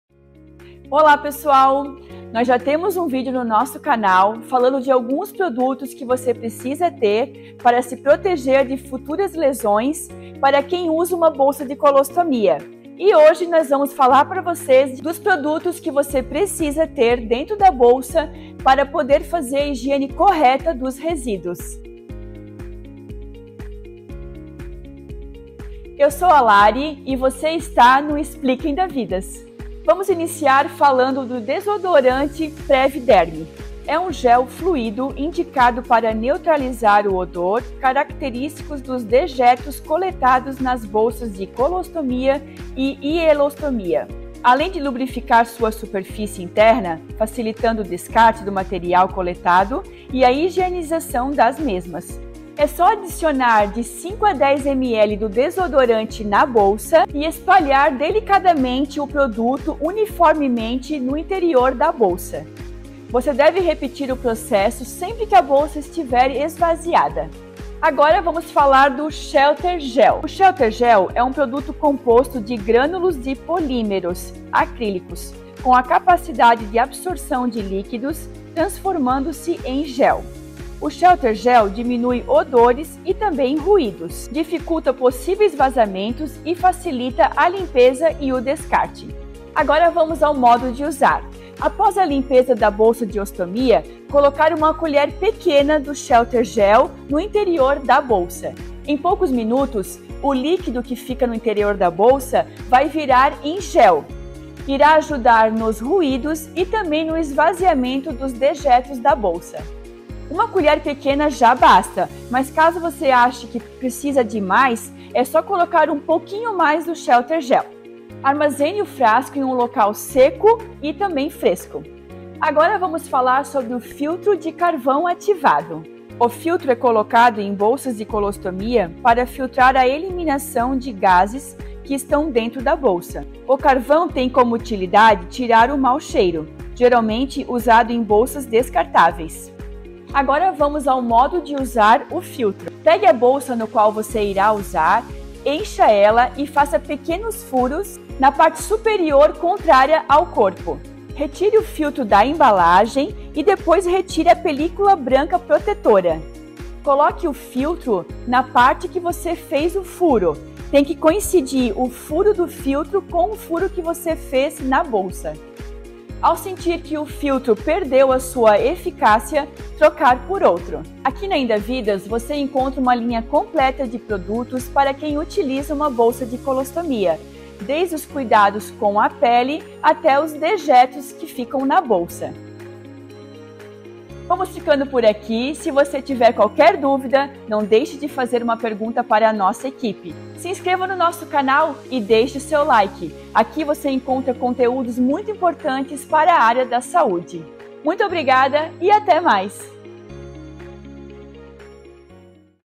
Bolsa de Colostomia - Produtos que Você Precisa ter para a Higienização Correta
Preparação da Pele para Bolsa de Colostomia: Dicas de Cuidados Essenciais

Se você ou alguém que você conhece usa uma bolsa de colostomia, é essencial saber como realizar a higienização de forma correta e segura.

Neste vídeo, vamos mostrar os produtos indispensáveis para manter a bolsa limpa e evitar irritações na pele, garantindo o conforto e a segurança necessários no dia a dia. A escolha dos itens certos faz toda a diferença na rotina de cuidados com a colostomia, e aqui você encontra informações essenciais para essa jornada.

Entenda mais sobre os tipos de produtos que auxiliam na limpeza, proteção e adesão da bolsa de colostomia, além de dicas práticas para um uso tranquilo e sem complicações.

Nosso objetivo é trazer informações valiosas que possam ajudar tanto quem utiliza a bolsa quanto quem cuida de alguém que a utiliza, promovendo qualidade de vida e bem-estar.